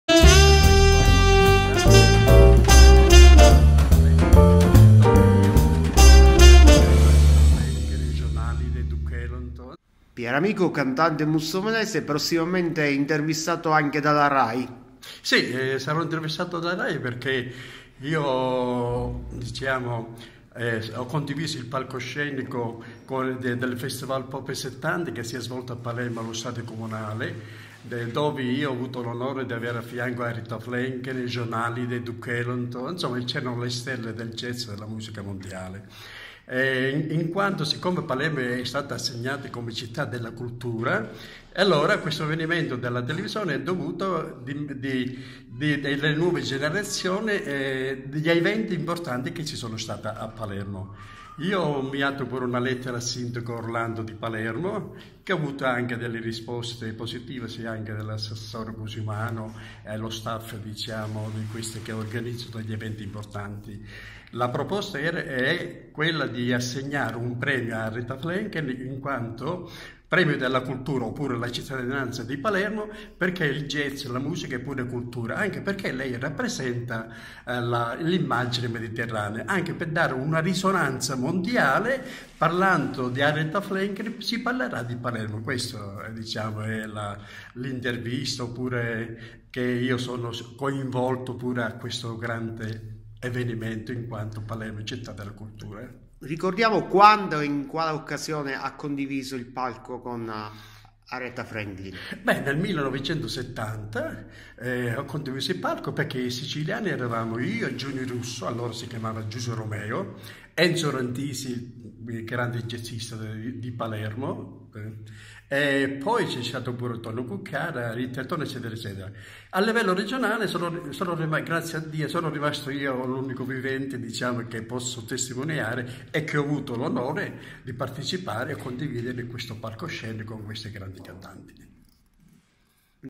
Piero Amico prossimamente in Rai
Il Cantante mussomelese a breve in Rai per parlare di Aretha Franklin con cui ha condiviso il palco a Palermo